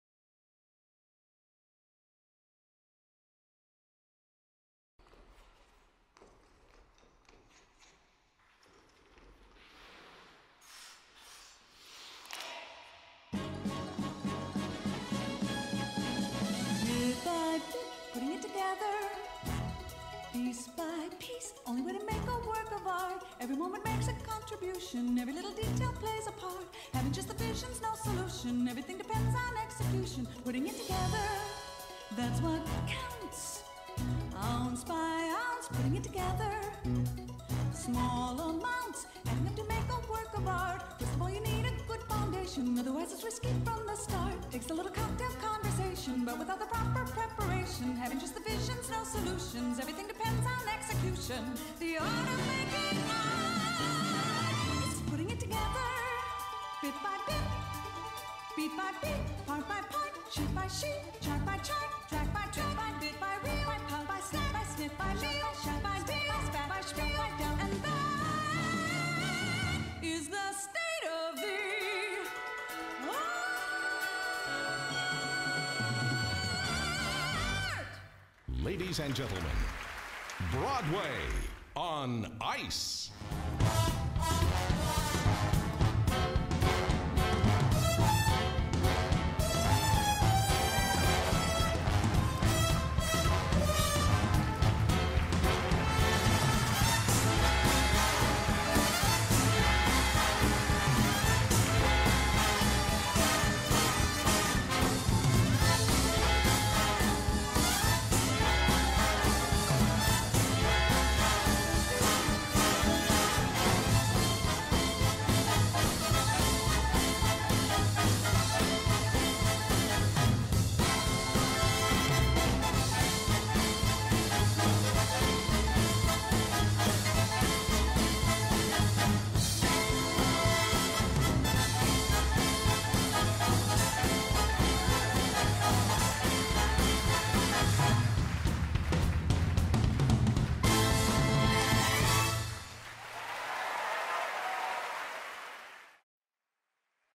Must Watch | The Making of Broadway on Ice - ROHM
This video features the stage preparations for the Broadway on Ice performance on Royal Opera House Muscat.